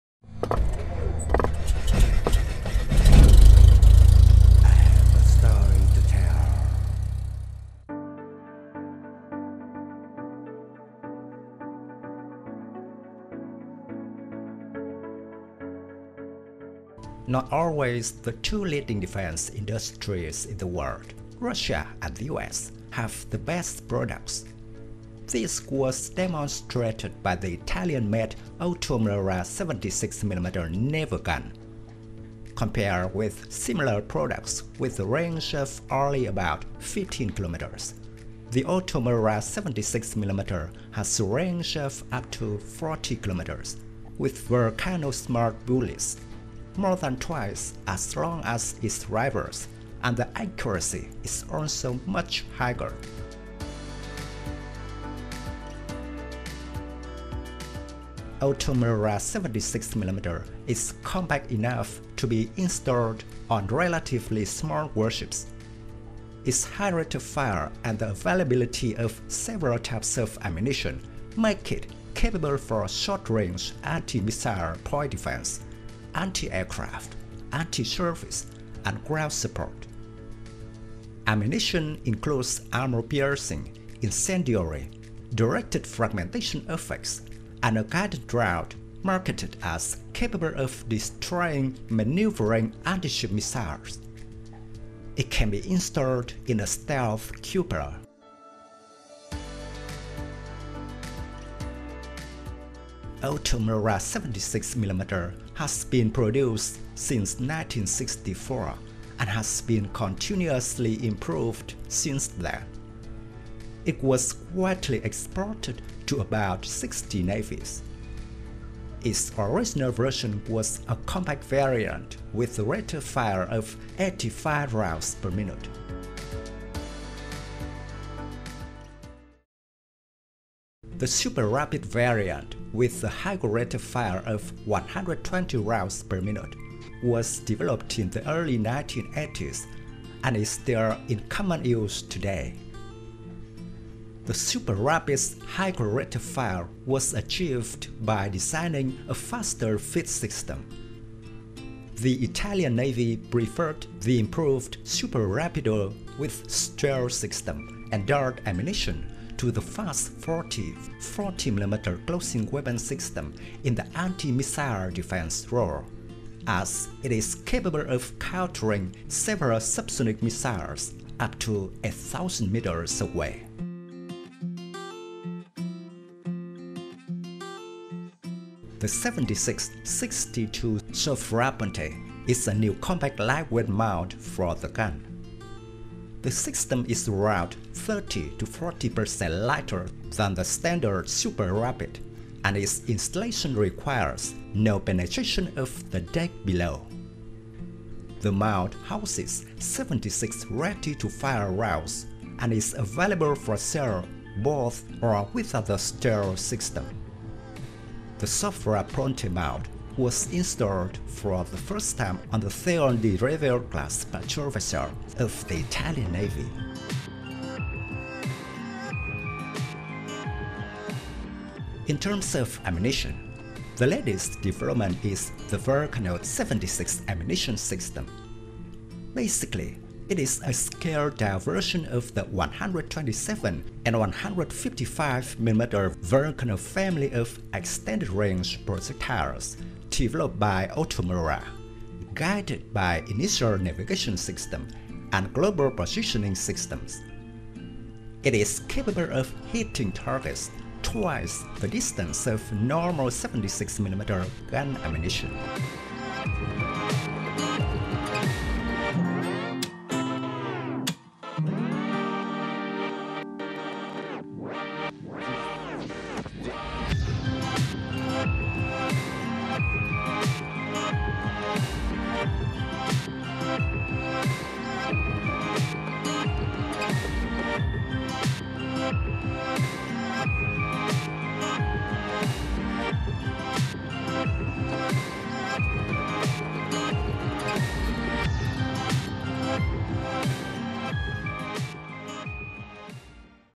OTO Melara 76 mm: The Most Powerful 76mm Naval Cannon In The World
the Italian-made OTO Melara 76 mm naval cannon. Compared with similar products with a range of only about 15km, the OTO Melara 76mm has a range of up to 40km with VULCANO smart bullets, more than twice as long as its rivals, and the accuracy is also much higher.

Thank you very much.
Best regards
Dung Tran.